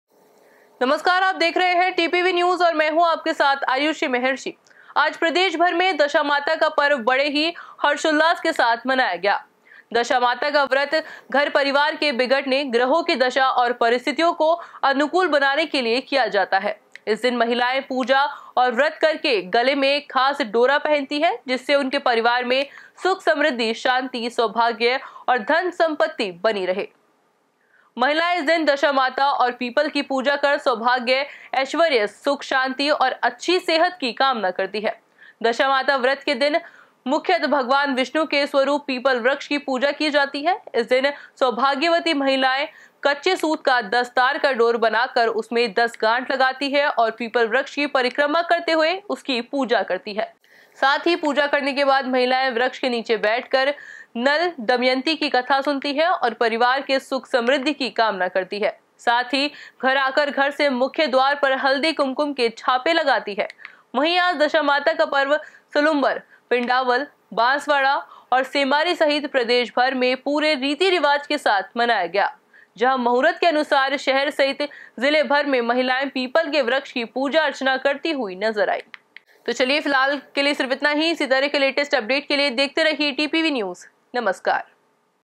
Dashamata Vrat 2022 | प्रदेशभर में मनाया गया दशा माता का पर्व | Dasha Mata Puja | TPV NEWS
प्रदेशभर में मनाया गया दशा माता का पर्व
पूरे विधि विधान के साथ की गई पूजा
महिलाओं ने की पीपल के वृक्ष की पूजा
सुख-समृद्धि और सौभाग्य के लिए की कामना